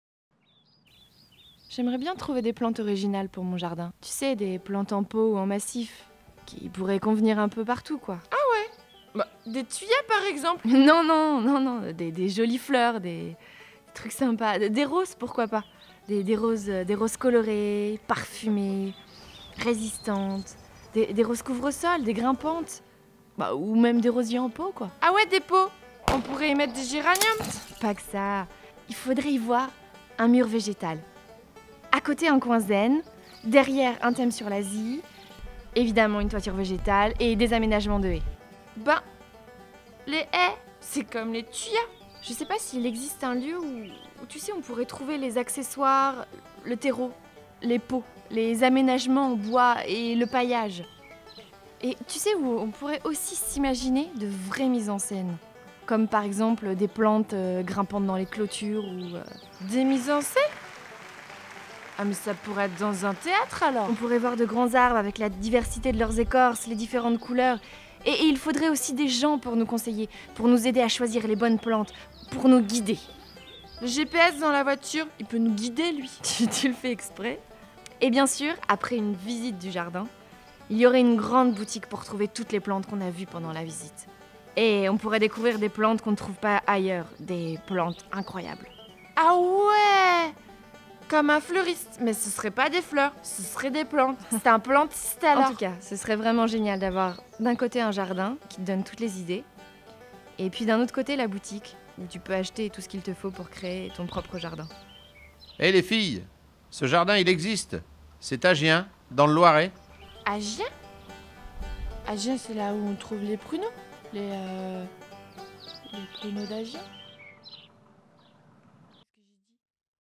Un jardin unique à GIEN
Venez découvrir un jardin qui propose des idées et des solutions pour agrémenter le votre, avec Globe Planter votre décorateur végétal.